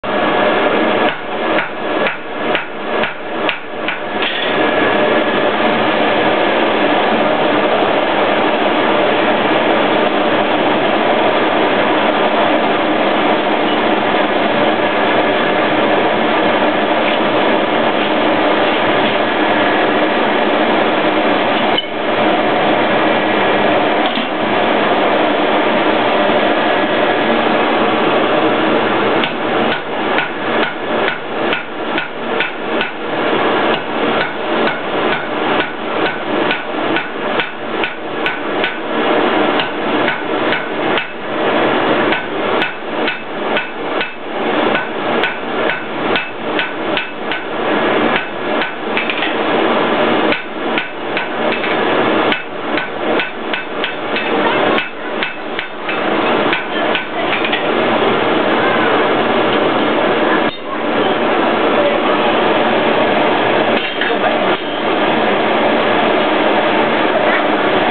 Blacksmith at Burwell Museum
Blacksmith demonstrating at Burwell Museum, Burwell, Cambridgeshire, England.
(sound is slightly out of sync for some reason)